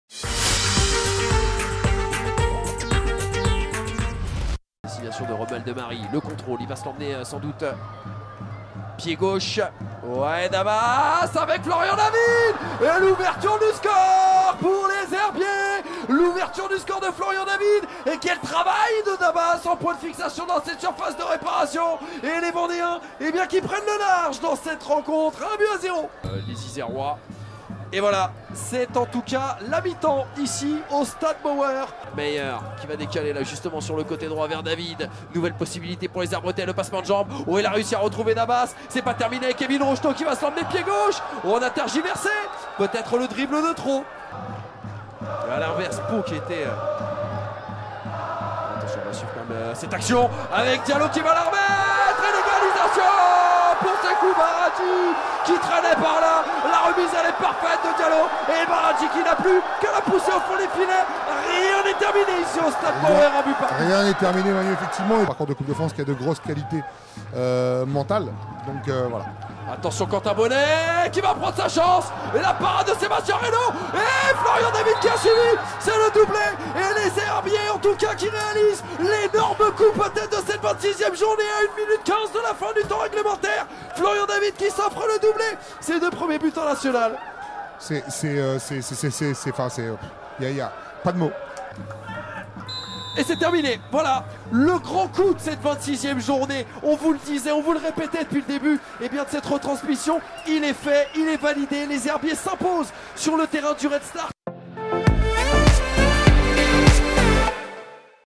J26 : Red Star FC - Les Herbiers VF (1-2), le résumé I National FFF 2018
Retrouvez les temps forts de la rencontre Red Star FC - Les Herbiers VF (1-2) comptant pour la 26e journée du National (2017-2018).
But pour le Red Star FC : Sekou Baradji (80e)
Buts pour Les Herbiers VF : Florian David (44e, 89e)